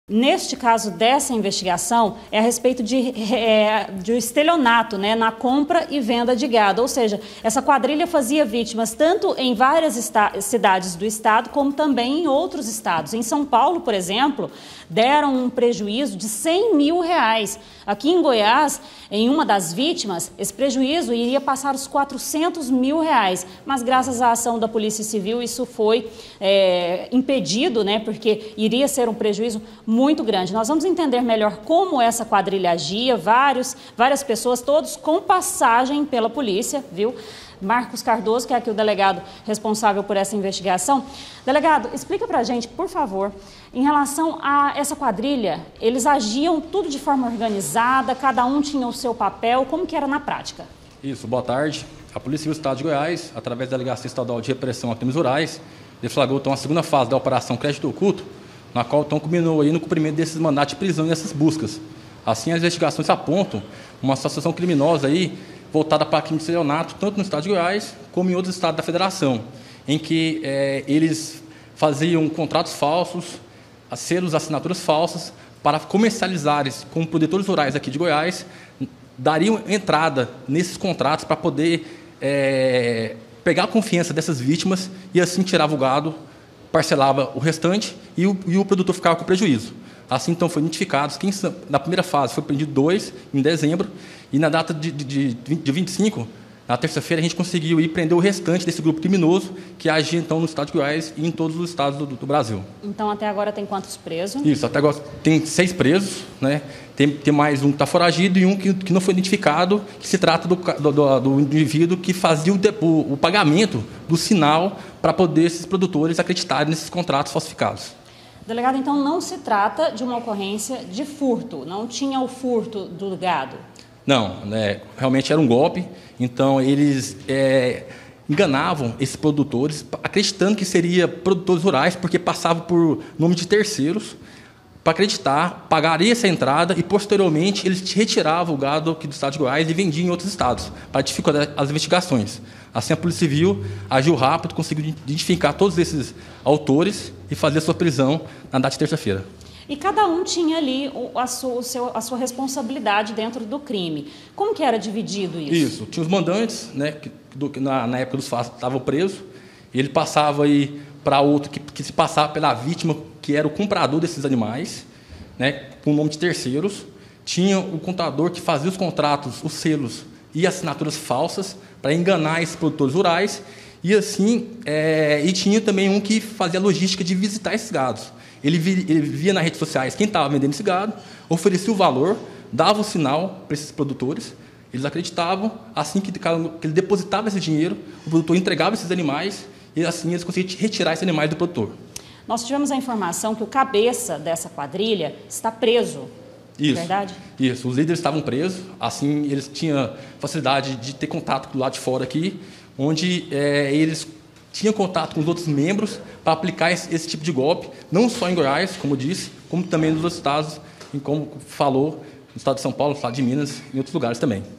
FRAUDE RURAL: QUADRILHA É INVESTIGADA POR COMPRA E VENDA DE GADO
PRESA QUADRILHA ENVOLVIDA EM FRAUDES NA COMPRA E VENDA DE GADO AQUI EM GOIÁS E EM OUTROS ESTADOS.